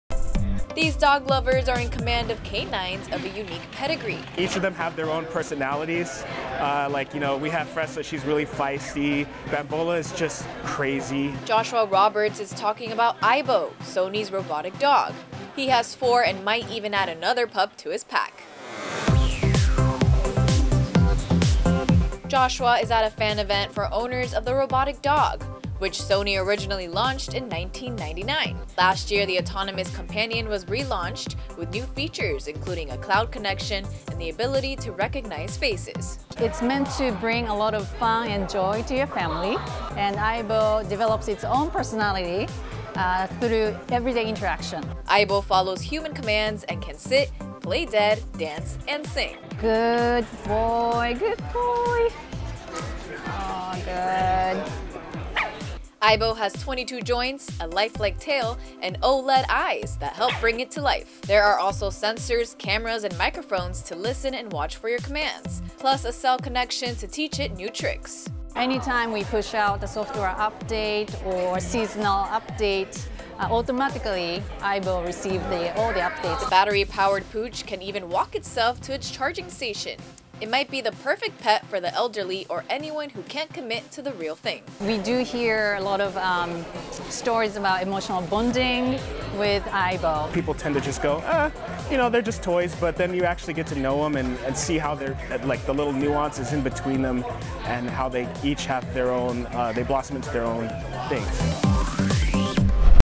Sony’s Robot Dogs
Sony first introduced it’s robotic dog - named Aibo - in 1999. Last year it got a relaunch with some new tricks! We went to an owner’s meetup where proud parents of these pooches shared a play date.